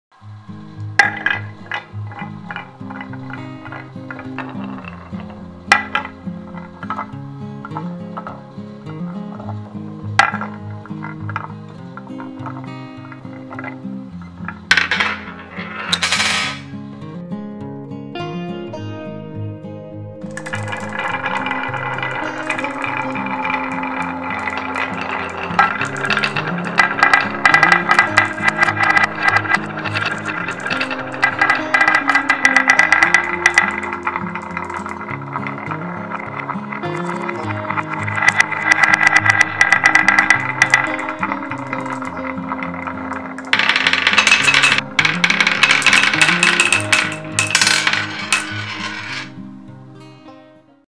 Marble Run Race ASMR & HABA Slope ☆ HABA Kugelbahn
Hello! This is a new series of videos Marble Run Race. Happy viewing!